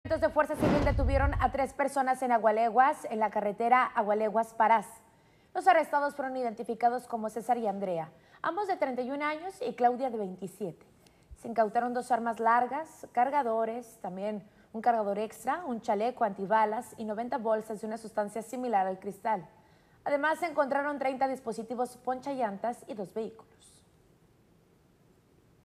Detienen a tres personas en Agualeguas, NL; aseguran armas de fuego, droga y vehículos
Programación
Lunes a viernes:
Telediario Matutino de 05:00- 10:00
Telediario Mediodía de 12:00-15:00
Telediario Nocturno de 19:00- 21:00